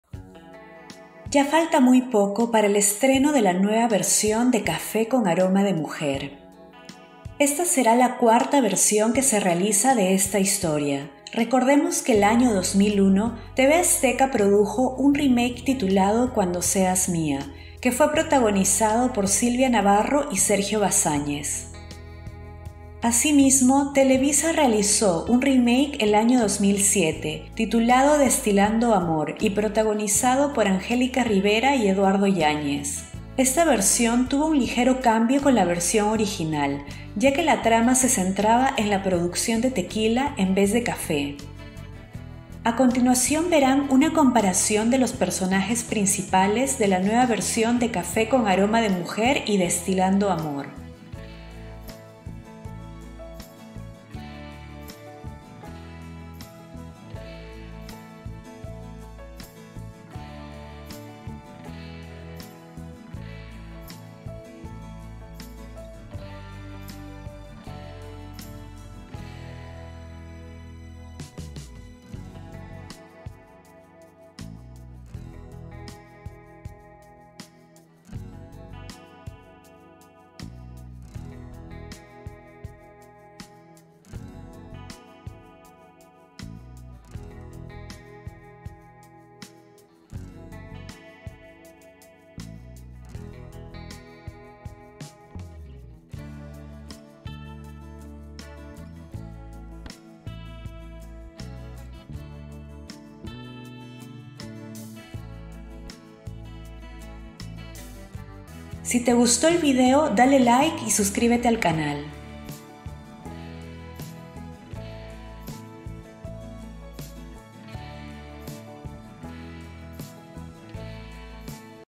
"Café con aroma de mujer" 2021 vs "Destilando amor"
Comparación del elenco de "Cafe con aroma de mujer" 2021, protagonizada por William Levy y Laura Londoño, y "Destilando amor", protagonizada por Angelica Rivera y Eduarno Yañez.

La nueva version se entrenara el 26 de mayo por Telemundo.